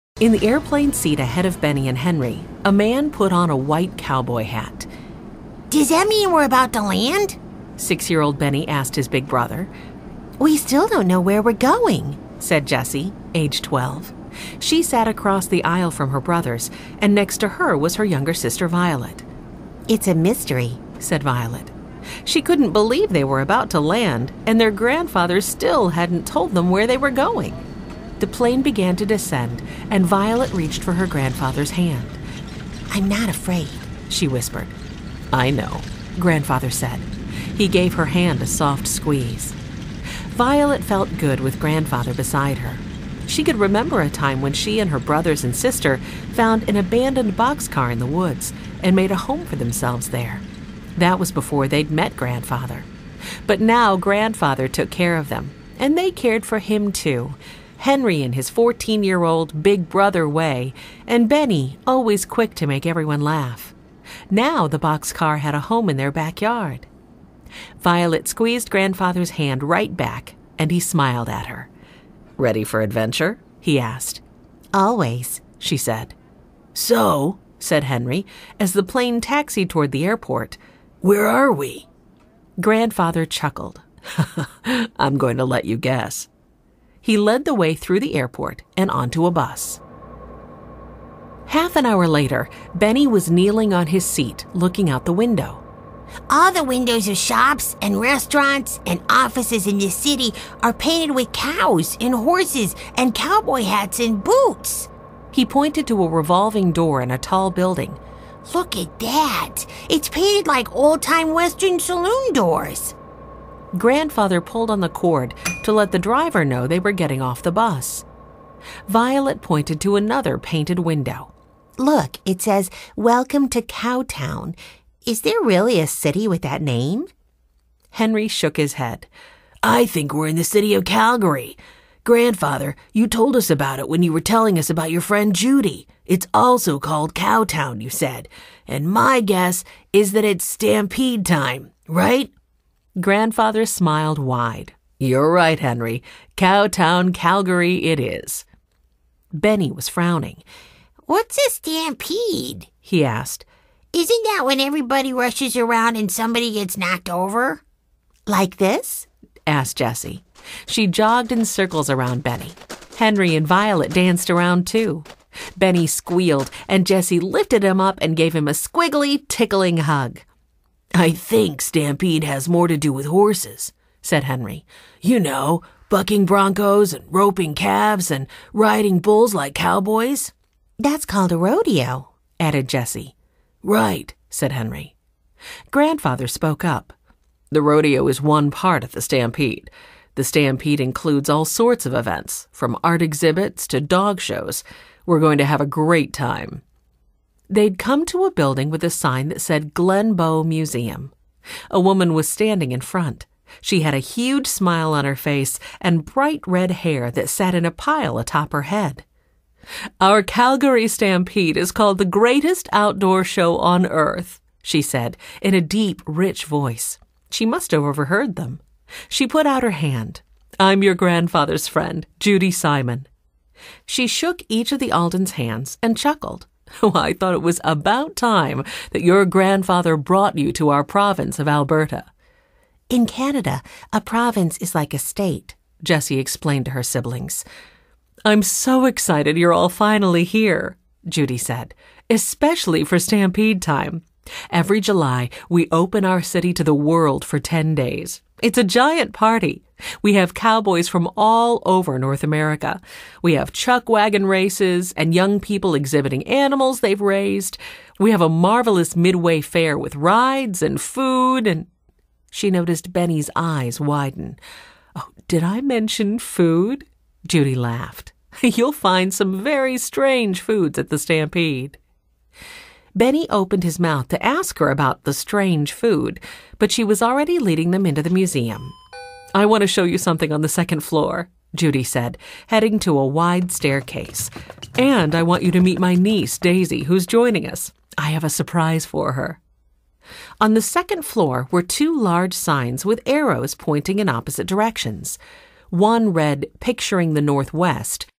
"The Mystery of the Calgary Stampede (Boxcar Children #140)" by Gertrude Chandler Warner
One warm night, four children stood in front of a bakery. No one knew them. No one knew where they had come from.